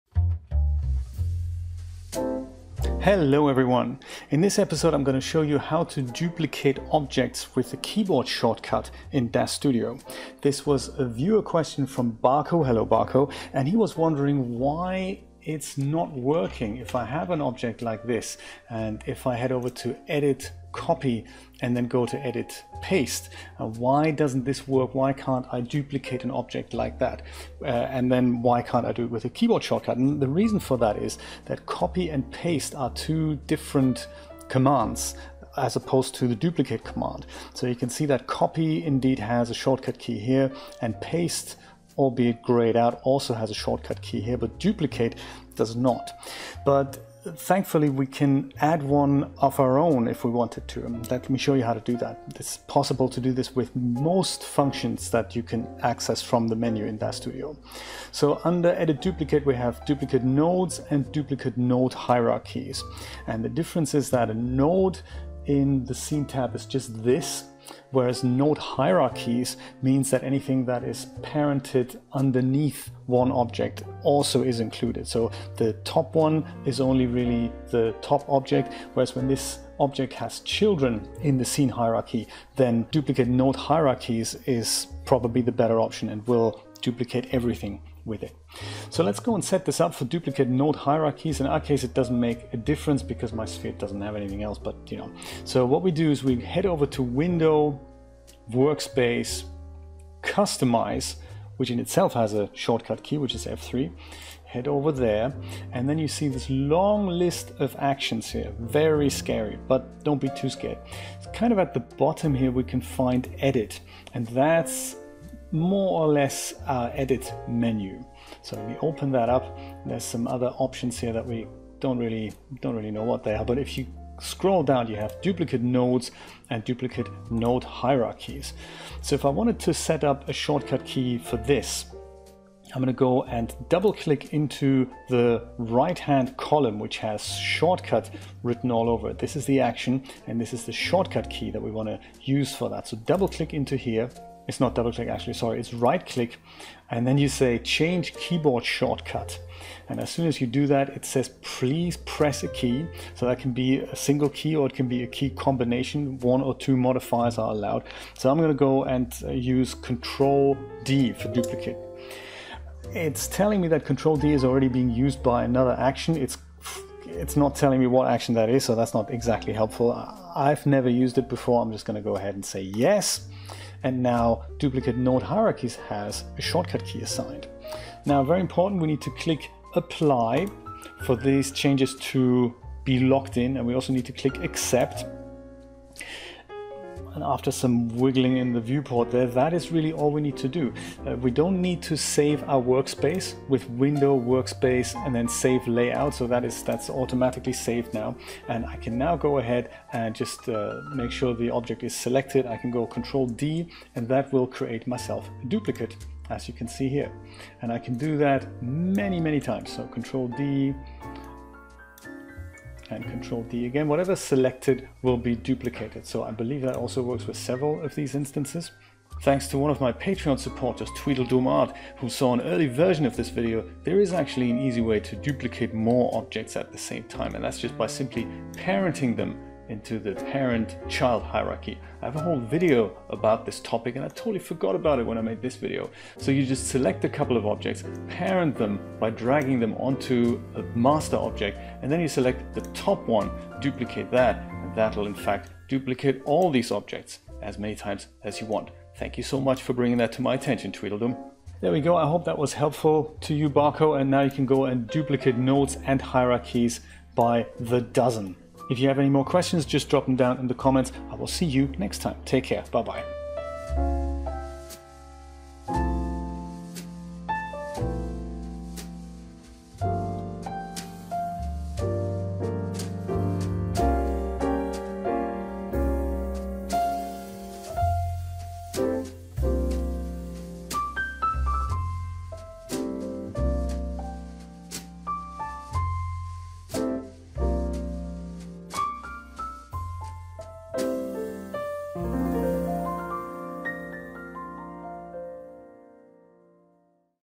Duplicating Objects with a Keyboard Shortcut in DAZ Studio
In this episode I’ll show you how to use a shortcut key to duplicate objects in your scene. DAZ Studio does not natively provide a shortcut for that, but we can set one up using the Customize Workspace option and potentially speed up workflow.

The shortcut key I used (CTRL+D) was already in use, and I didn't know what it was associated with. I've since found out: it was "Edit - Figure - Drop to Floor". Quite useful, so don't override that like I did!

This is a new segment on my channel for questions I get that are answered easier as a video than as a comment. I hope this was helpful.

For Barco.